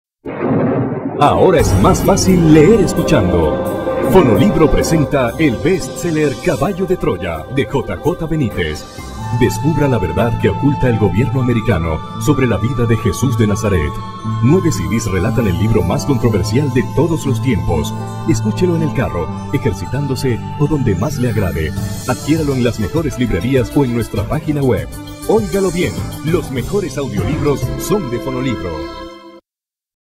Caballo de Troya - Jerusalen
Jerusalen, Caballo de Troya audiolibro dramatizado en espanol, basado en el bestseller international del aclamado escritor J.J. Benitez